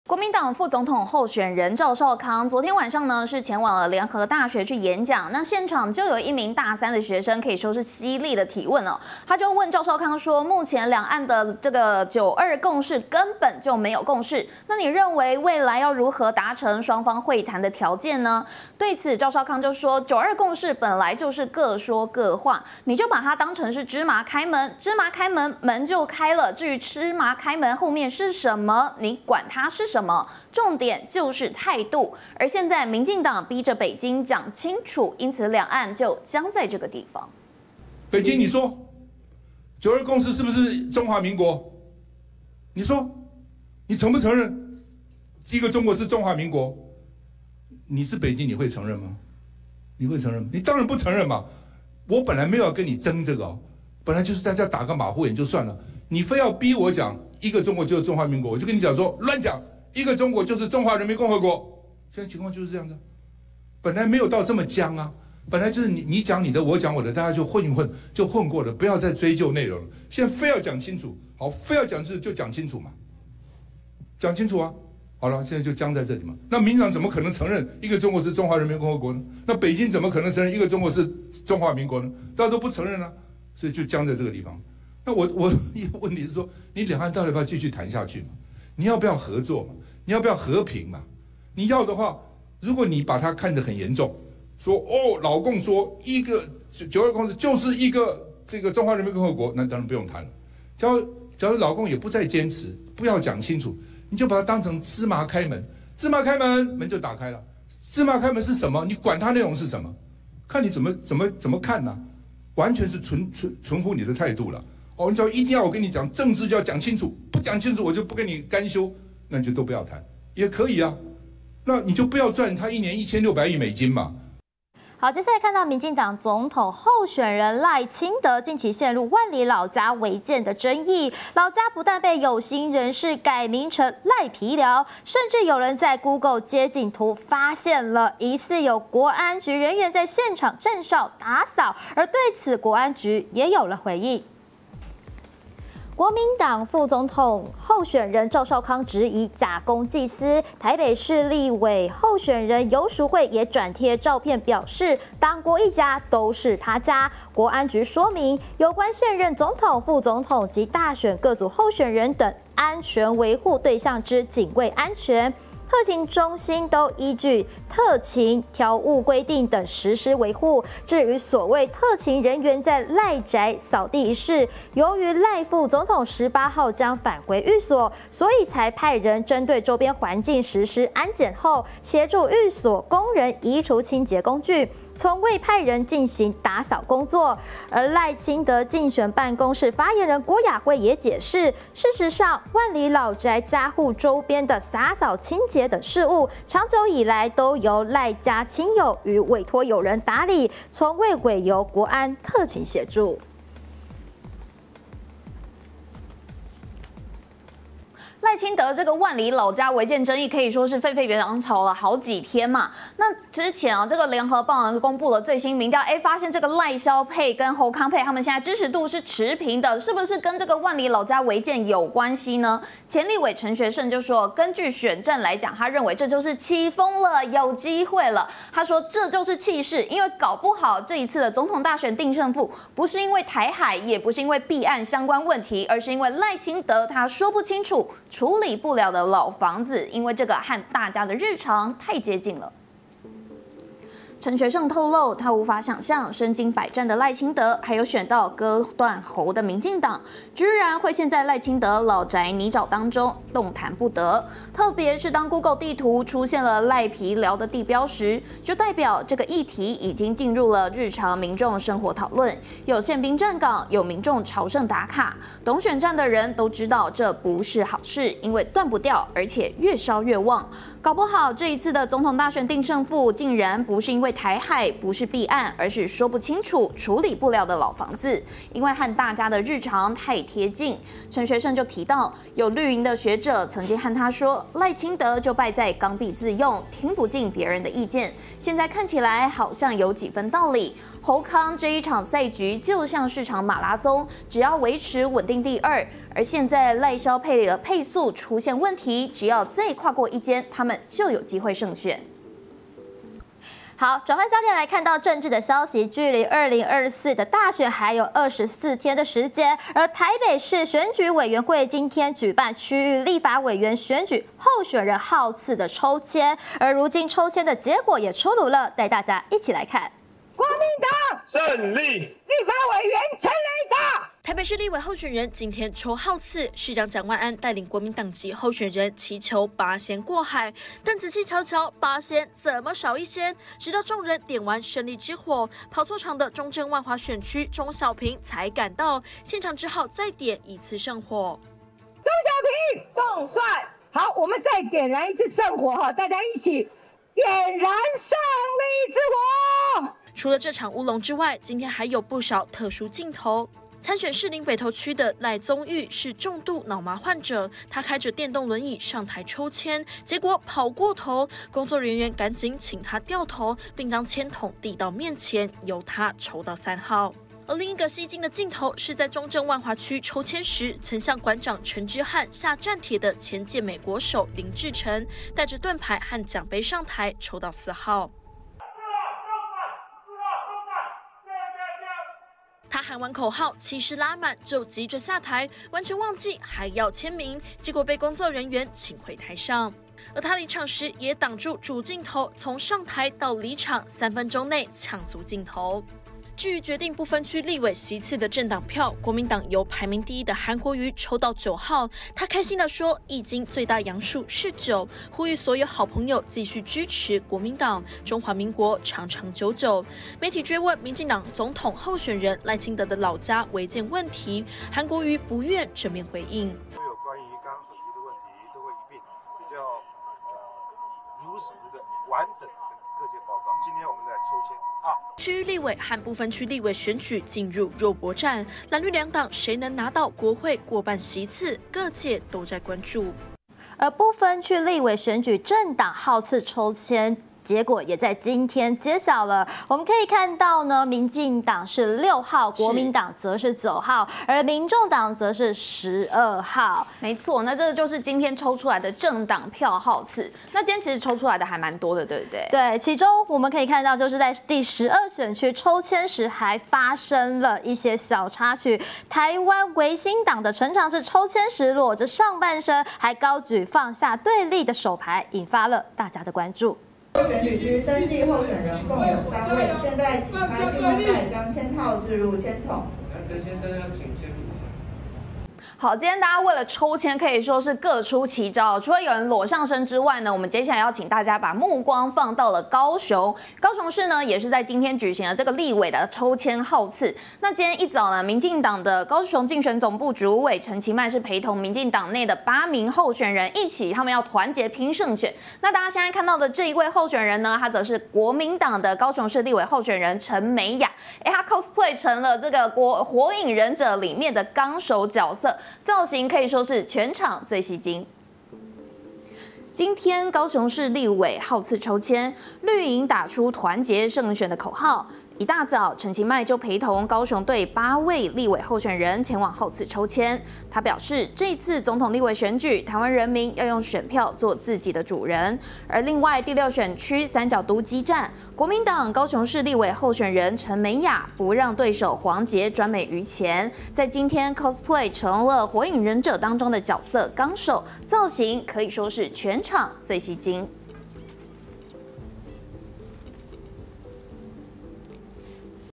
趙少康談九二共識：當成芝麻開門｜20231220 ET午間新聞
主播 黃凱翊、邱筱茜

新聞內容
趙少康談九二共識：當成芝麻開門
賴老家「特勤清掃」照瘋傳 國安局：僅維安
陳學聖看大選：起風了！「賴皮寮」定勝負
立委號次抽籤 候選人各出奇招
政黨票號次 民進黨6國民黨9民眾黨12
高雄立委抽籤 她cos火影綱手吸睛